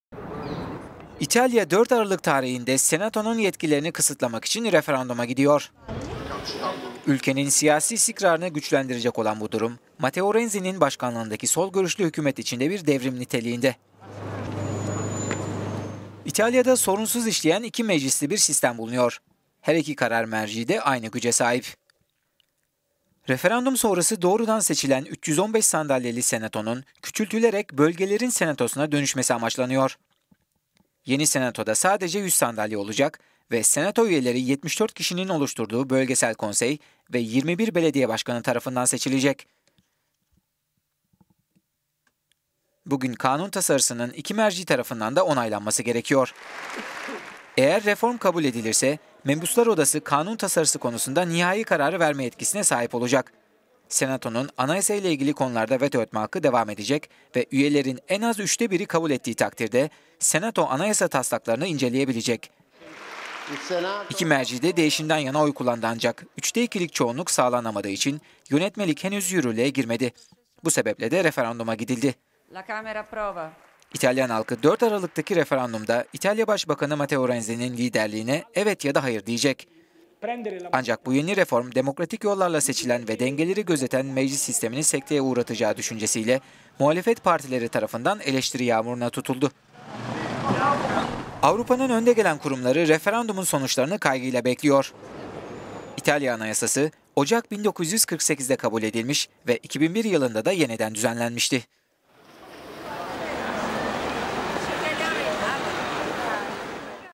Herkes 4 Aralık'a kilitlendi, İtalya tarihi referandumun sonucunu bekliyor
4 Aralık tarihinde İtalyanlar senatonun yetkilerini kısıtlamak için referanduma gidecek.

Ülkenin siyasi istikrarını güçlendirecek olan bu durum, Matteo Renzi'nin başkanlığındaki sol görüşlü hükumet için bir devrim niteliğinde...

İtalya'da sorunsuz işleyen iki meclisli bir sistem bulunuyor. Her iki karar merci de aynı güce sahip.

Referandum sonrası doğrudan seçilen 315 sandalyeli senatonun, küçültülerek 'bölgelerin senatosu'na dönüşmesi amaçlanıyor.